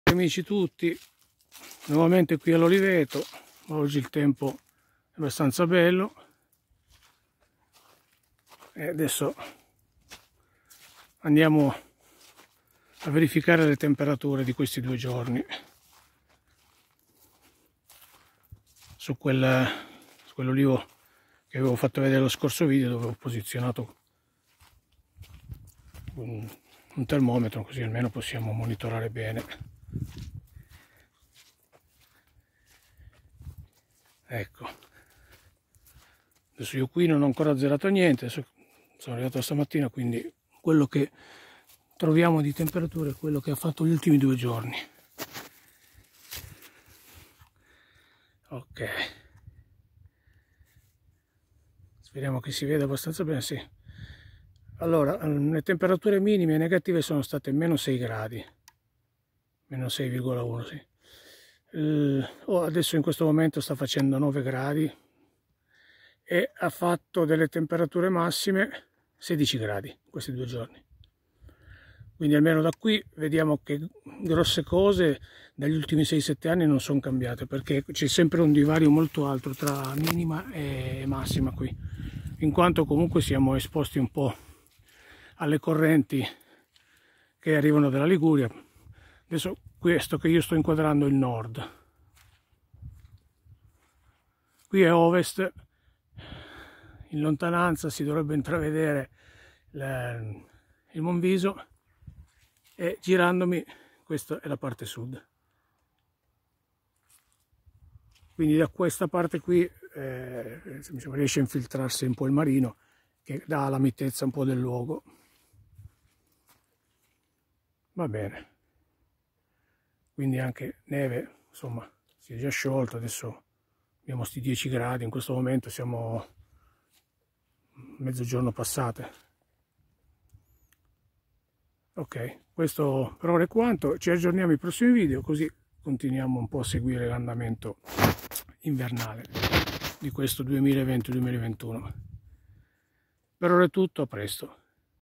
Nell'Oliveto che tempo che fa ?❄💪💪💪
Oggi 30/12/2020 aggiorniamo le temperature che registrano sostanzialmente uno sbalzo termico da -6 gradi a più 16 che rientra nelle rilevazioni degli ultimi 7/8 anni comunque seguitemi che vedremo l'evoluzione di questo inverno.